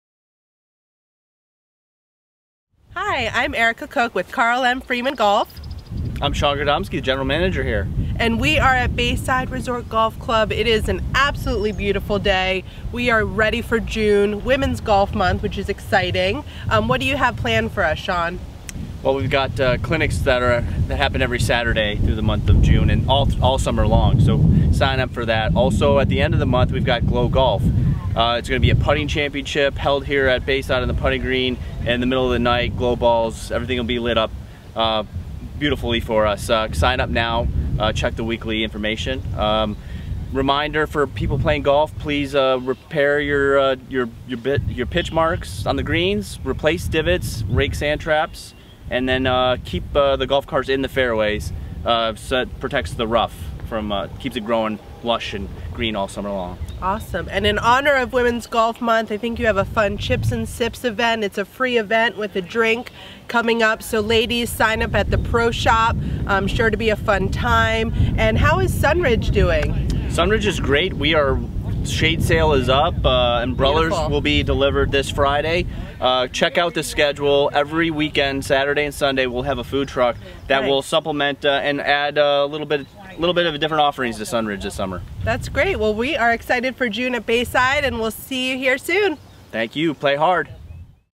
Bayside Fenwick Island General Manager Update June 2018
June is Women's Golf Month! Sean and Erika update us on some of the exciting events planned including Chips & Sips, which includes instruction and a free drink for the ladies as well as Glow Golf, which is sure to be a great time for the family.